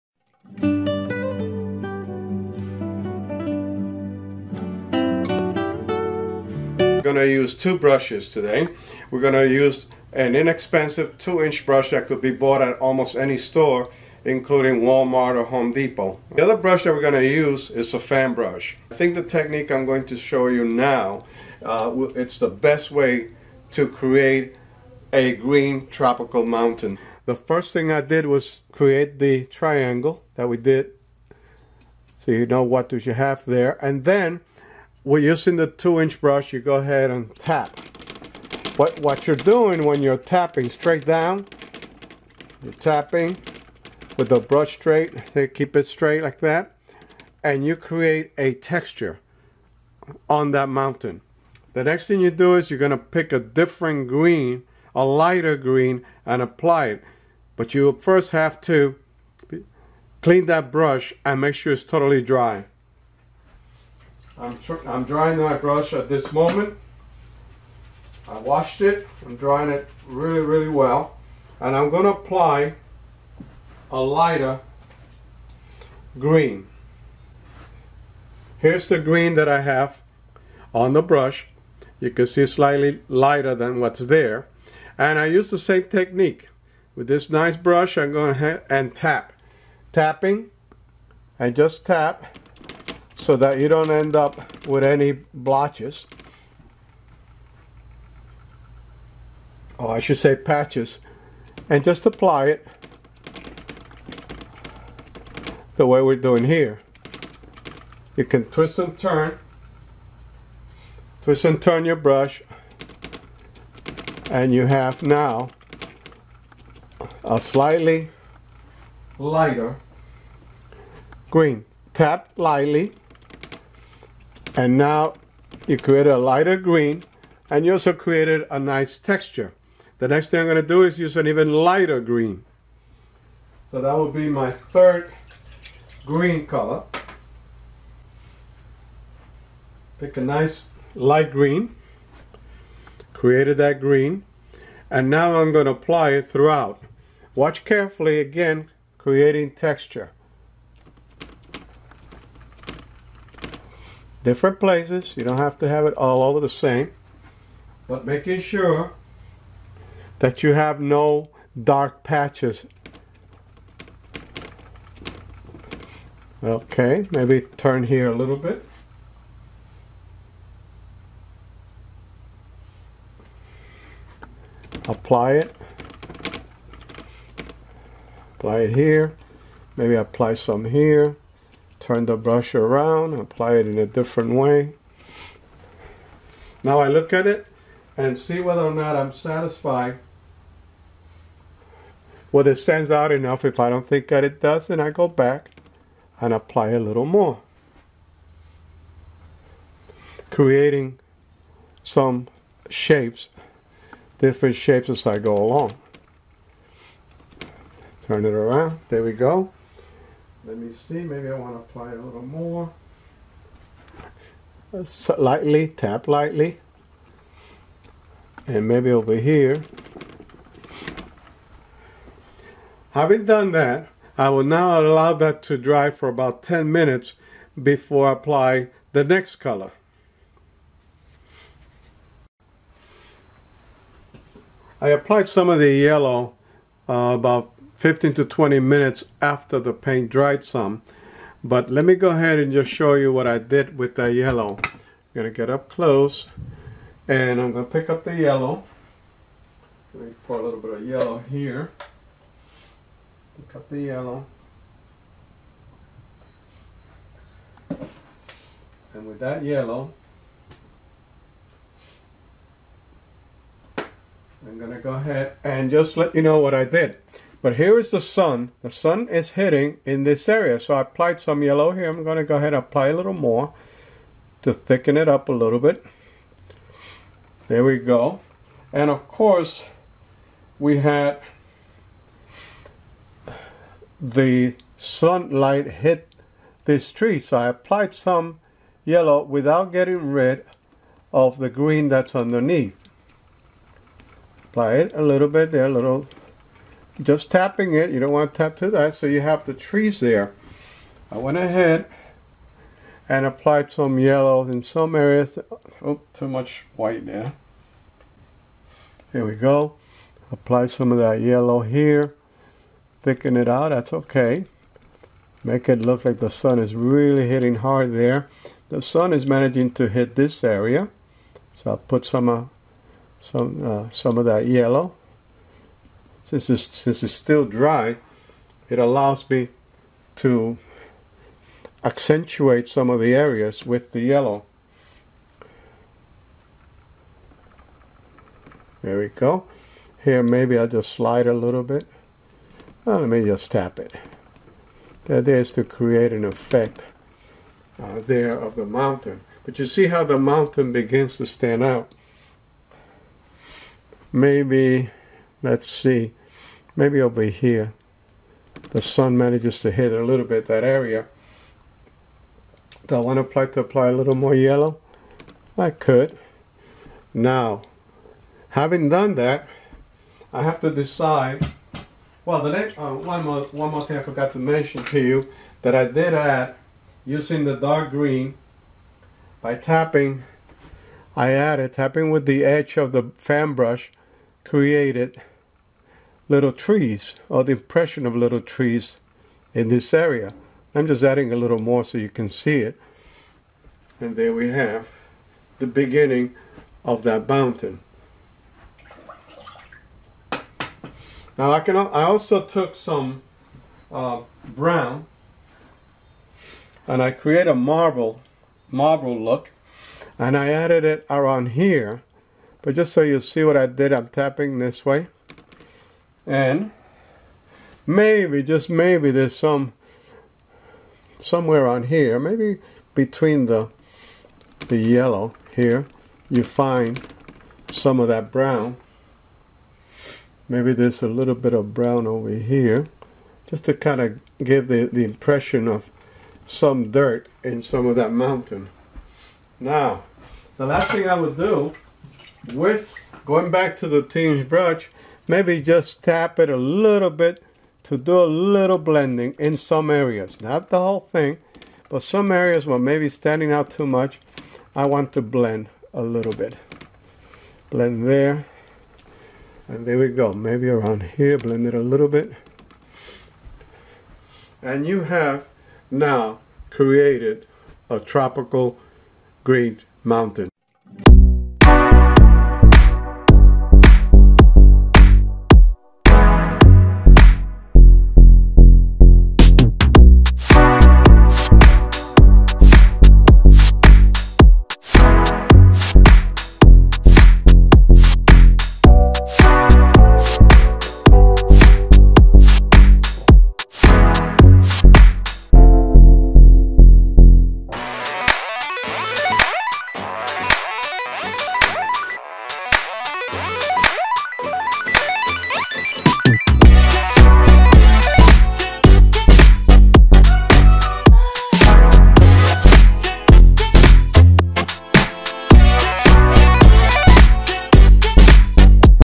HOW TO PAINT GREEN TROPICAL MOUNTAINS
The artist uses two brushes, shapes and layers to create a simple green tropical mountain.